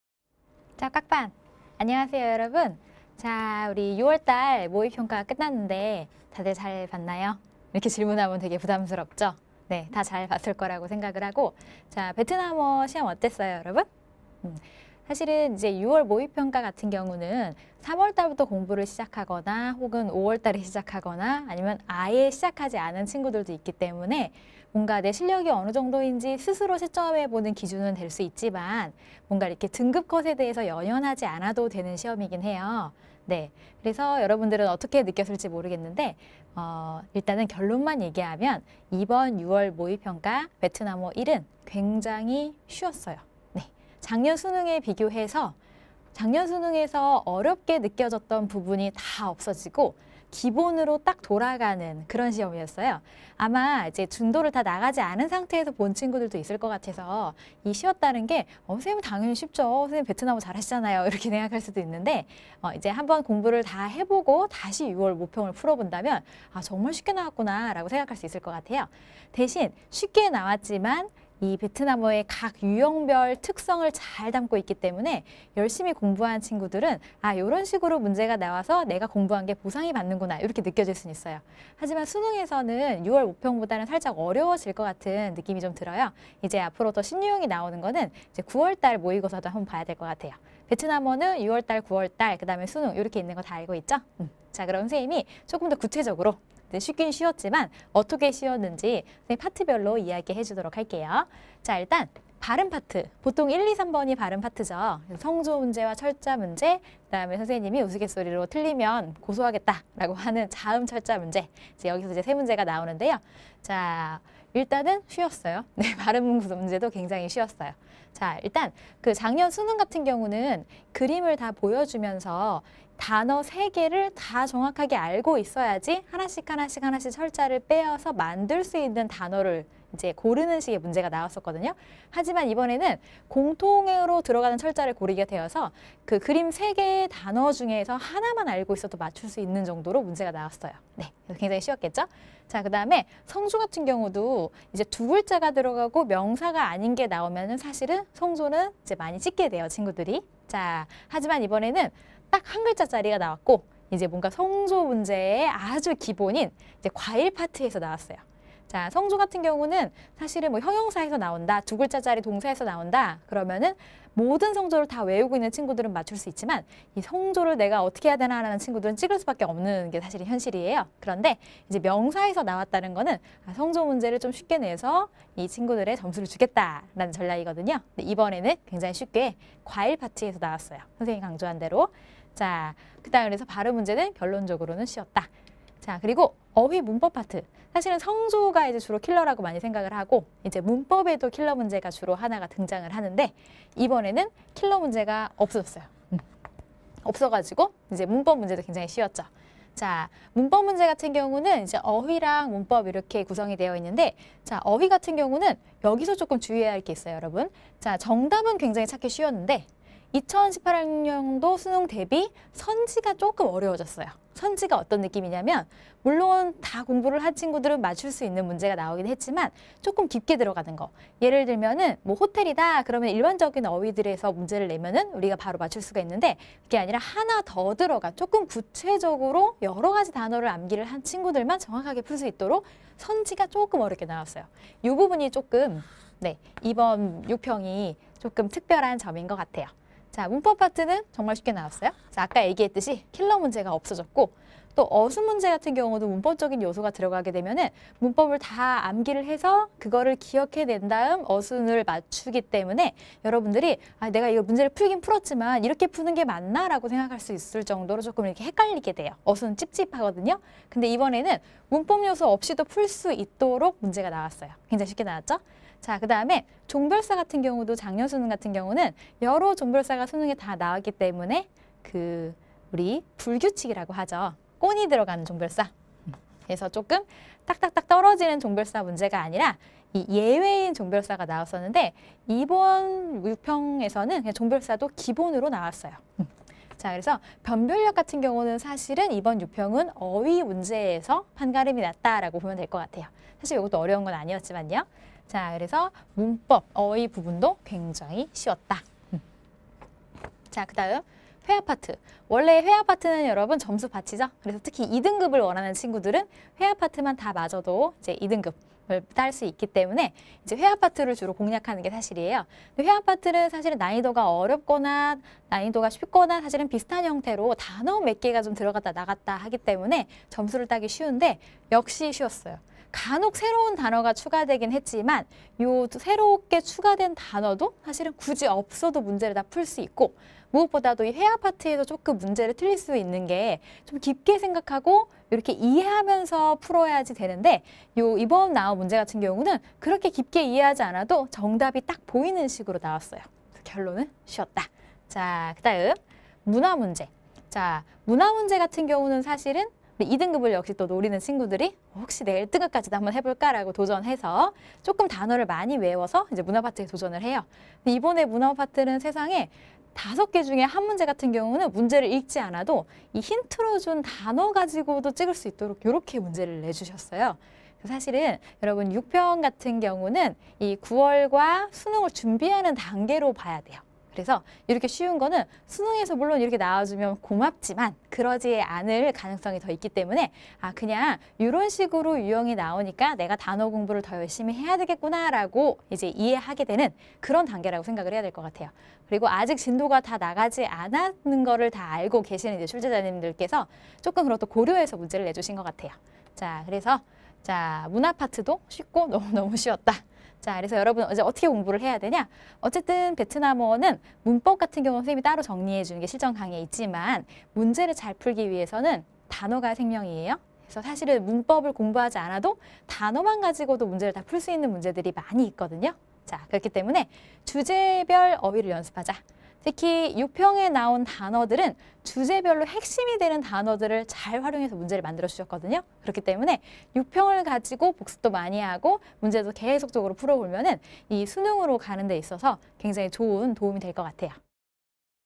[메가스터디] 베트남어 홍빛나쌤 - 6월 모평 베트남어 총평
[6월 모의고사] 평이했던 베트남어 총평

- 발음 파트 : 작년 수능 대비 쉬운 구성
- 어휘 파트 : 정답은 찾기 쉬우나, 작년 수능 대비 어려워진 선지
- 문법 파트 : 작년 수능 대비 쉬운 구성
- 회화 파트 : 쉽고 기본적인 부분에서 출제
- 문화 파트 : 쉽고, 쉽고 너무너무 쉬웠다!

문제를 잘 풀기 위해서는 단어가 핵심입니다.
6평 문제를 꼼꼼하게 분석하고, 반복적으로 문제 풀이 연습을 합시다!

빛나는 수능 베트남어 홍빛나 선생님이 궁금하다면?